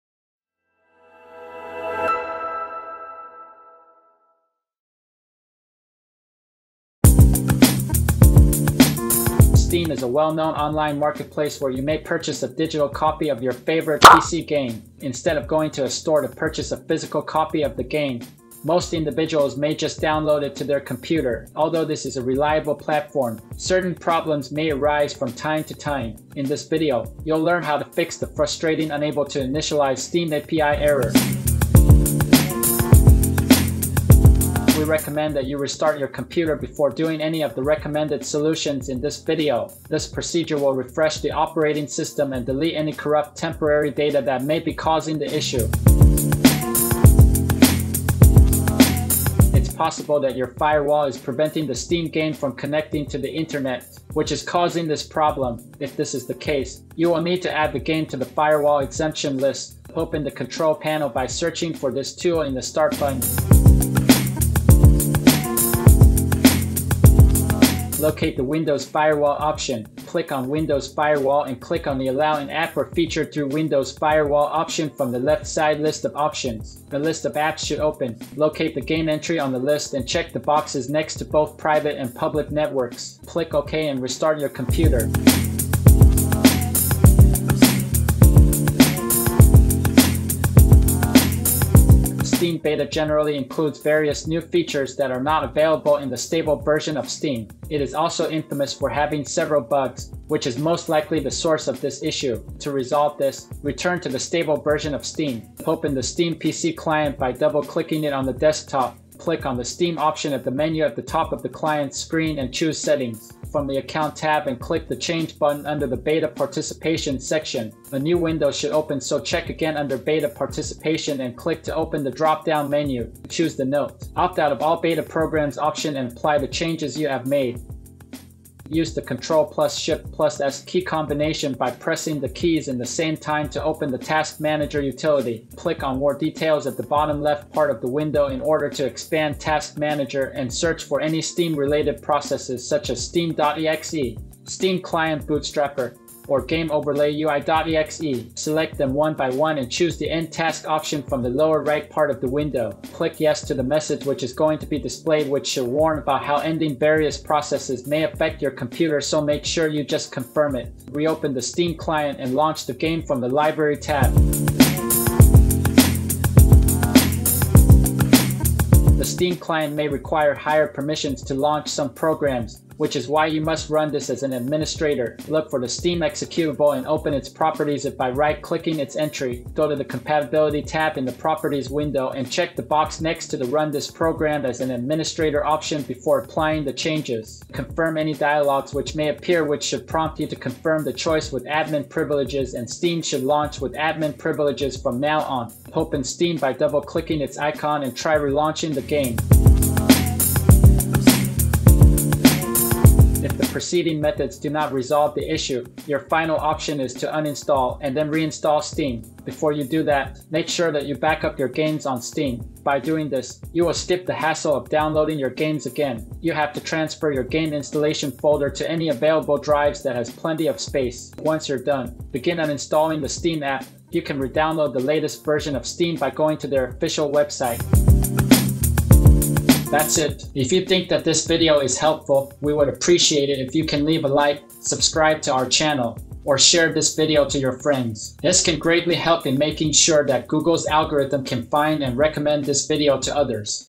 SOLVED: Unable To Initialize Steam API Error | Easy Solutions
Steam is a well-known online marketplace where you may purchase a digital copy of your favorite PC game. Instead of going to a store to purchase a physical copy of the game, most individuals may just download it to their computer. Although this is a reliable platform, certain problems may arise from time to time. In this video, you’ll learn how to fix the frustrating “Unable To Initialize Steam API” error.